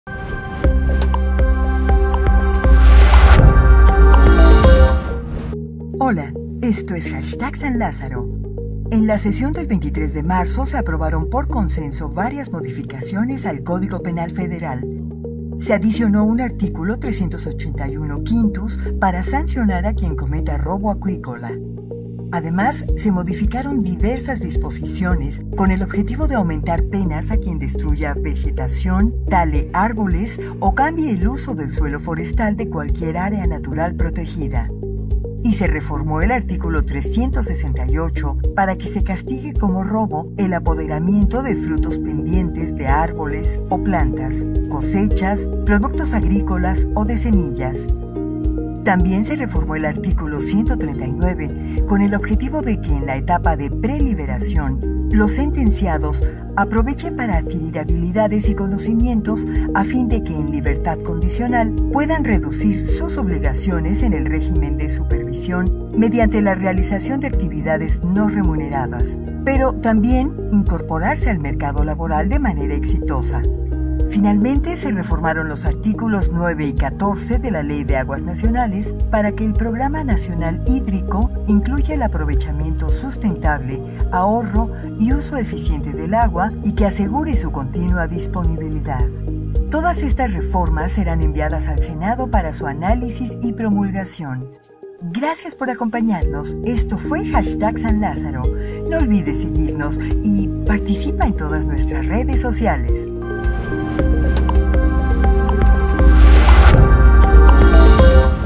En SanLázaro aprobamos reformas por consenso.
🟢 En SanLázaro, aprobamos reformas por consenso para

🔹Sancionar a quien cometa robo acuícola.
🔹Eliminar la frase "de forma exclusiva" en el status de personas sujetas a libertad condicional.
🔹Que el Programa Nacional Hídrico incluya el aprovechamiento sustentable, ahorro y uso eficiente del agua.